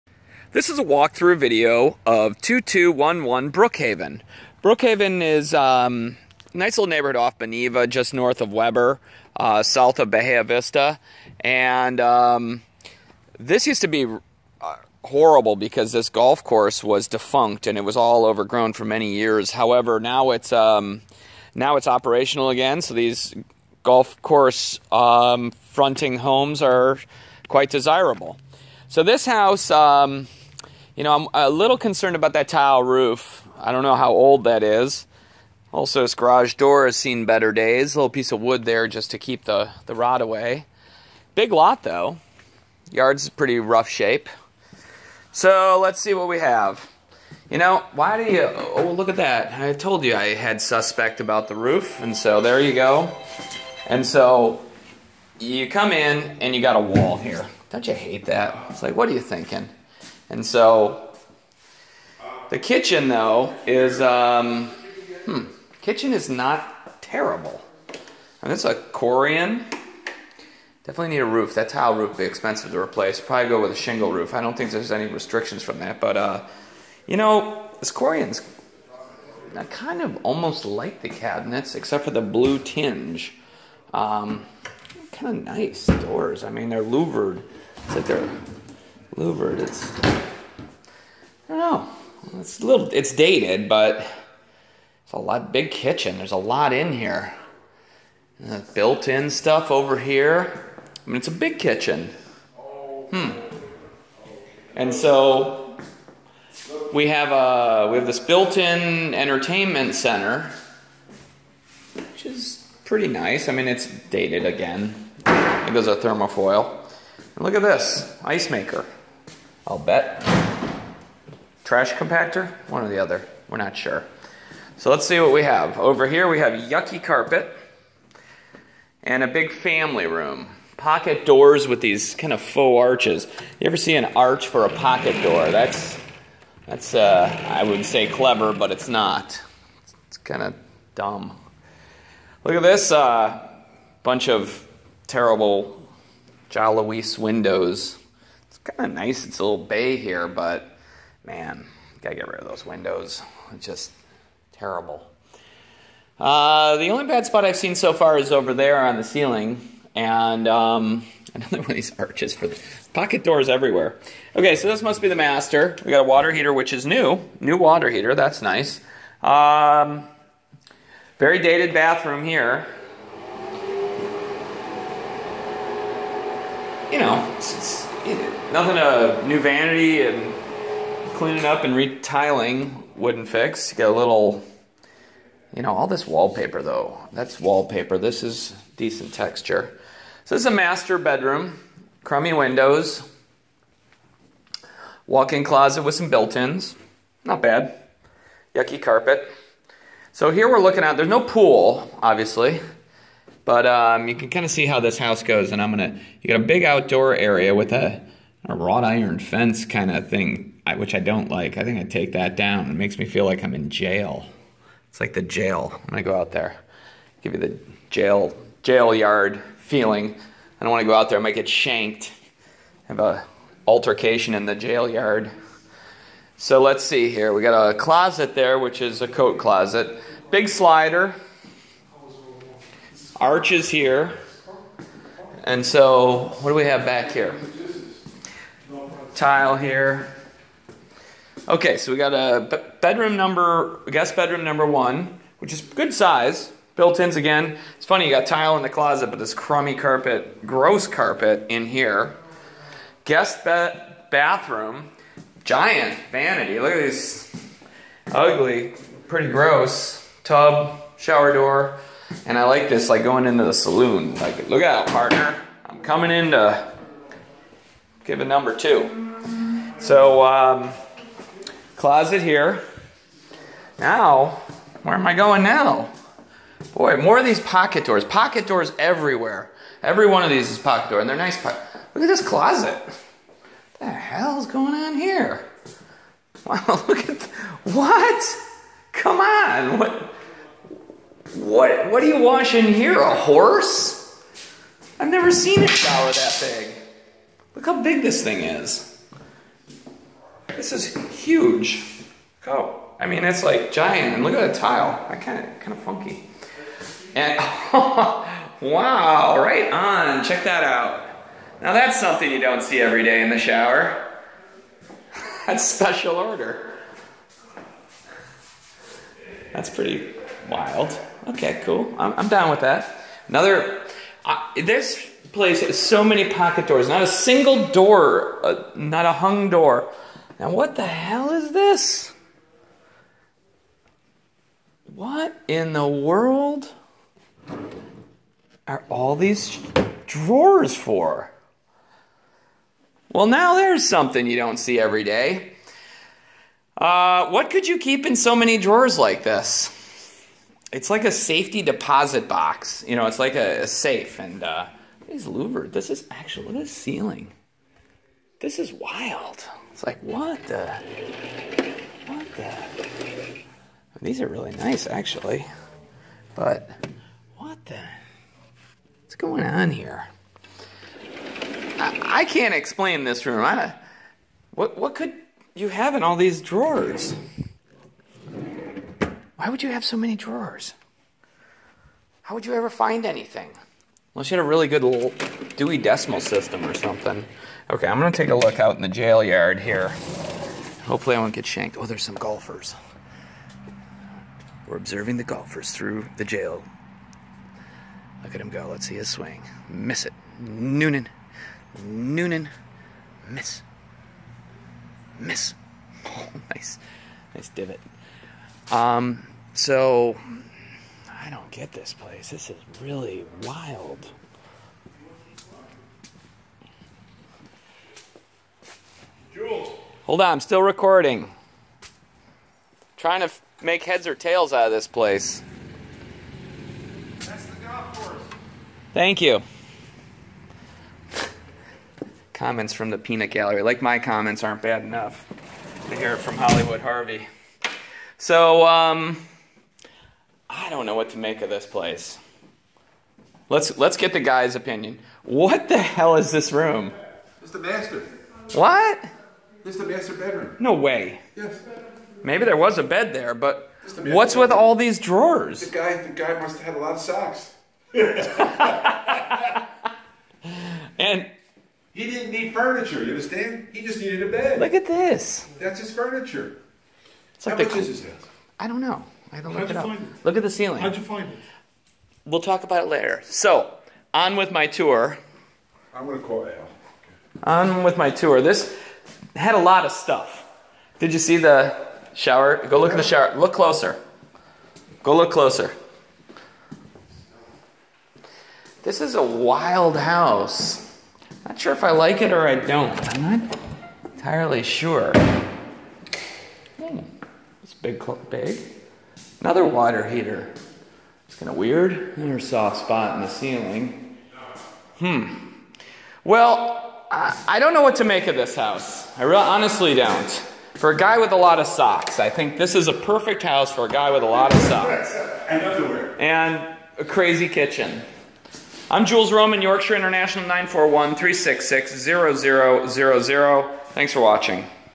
2211 BROOKHAVEN DR, SARASOTA, 299K
County: Sarasota Status: Active
Subdiv: FOREST LAKES COUNTRY CLUB ESTATES
Baths: 3/0 Special Sale: REO/Bank Owned
Pool: None ADOM: 35
Style: Single Family Home CDOM: 35
Location: Close to Bus Line, Golf Course View, In County, Street Paved
Total Acreage: 1/4 Acre to 21779 Sq. Ft. Pets: Yes
Minimum Lease Period: No Minimum Max Times per Yr:
Garage/Carport: 2 Car Garage, Attached, Door Opener, Side Rear Entry, Washer/Dryer Hookup

Cute three bedroom three bath home in Forest Lakes Country Club Estates. Separate breakfast room, solid surface counters, breakfast bar. Oversized lot overlooking the new Palms Golf Course. Centrally located near shopping centers, restaurants, schools, beaches and main roadways.